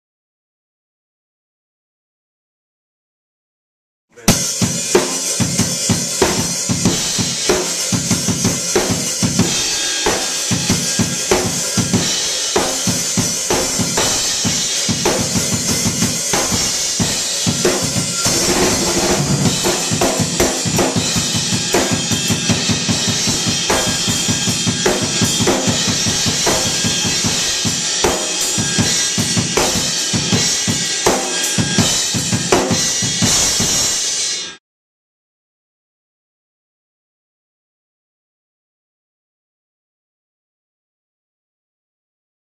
Great Gray Buildings: Recording Drums Day 1
Matt does dumb stuff on his drums.